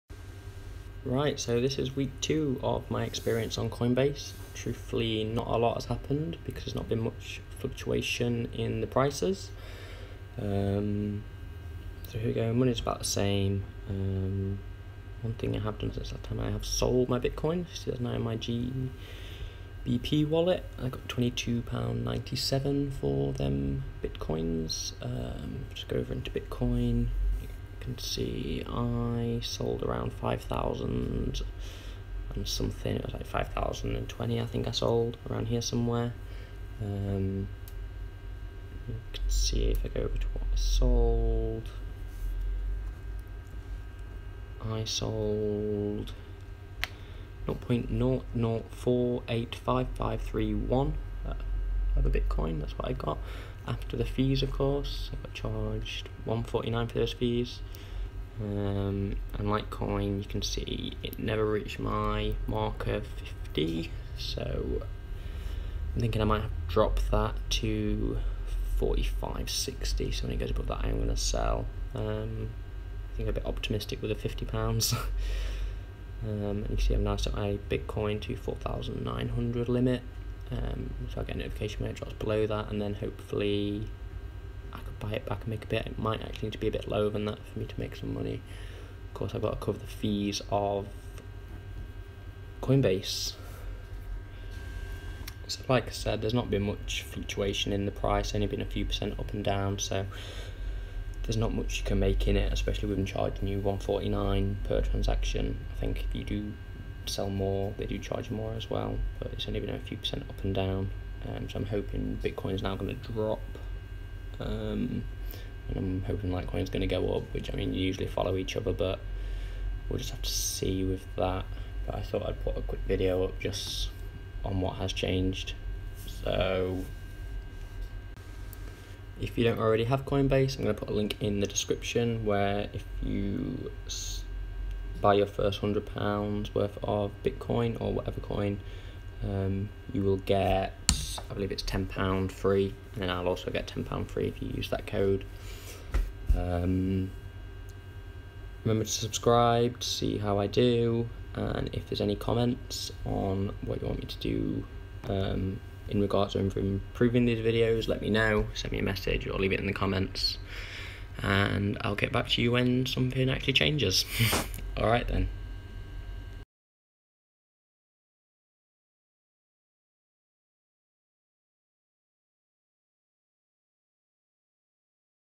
Can you make money with Coinbase / bitcoin week 2
Week 2 17th September 2018
Can I make money by buying and selling bitcoin.
I am not a professional trader I just want to see if anyone can make money using Coinbase with very little knowledge of where the market is going to go.